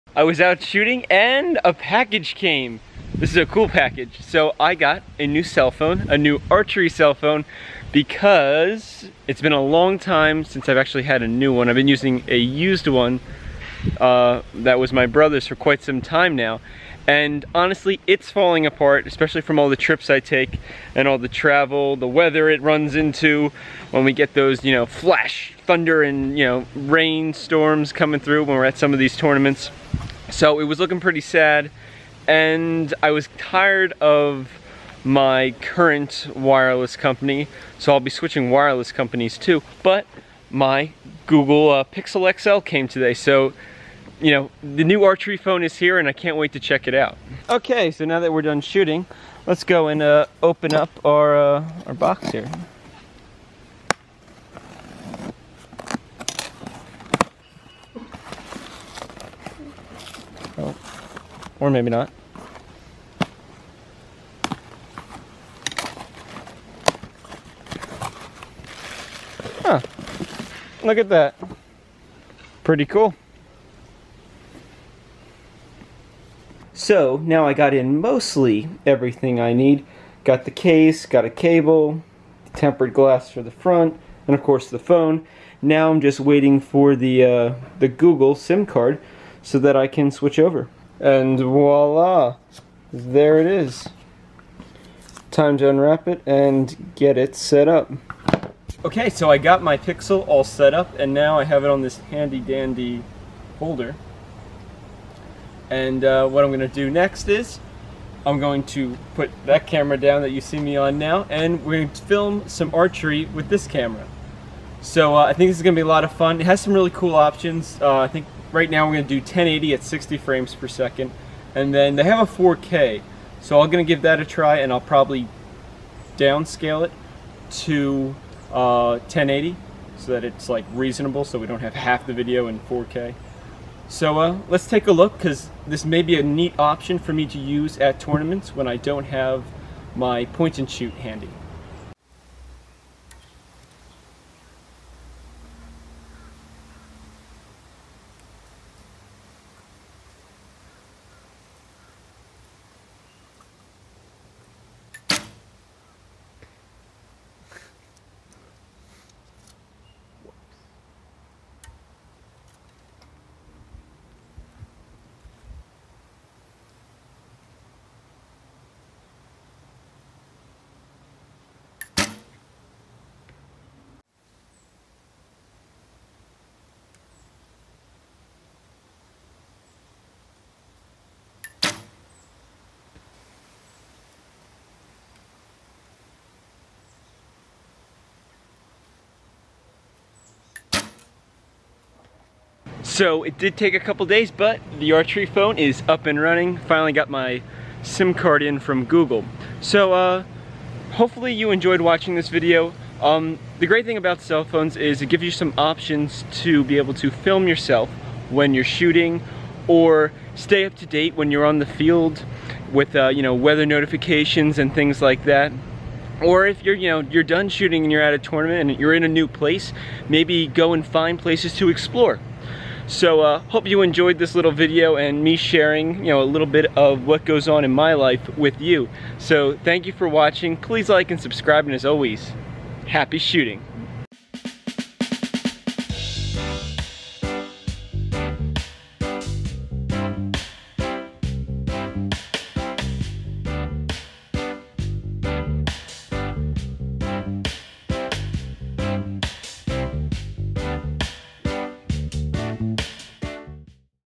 My New Archery Phone Pixel XL
After years of use and many archery tournaments but wet and dry my new archery phone has arrived!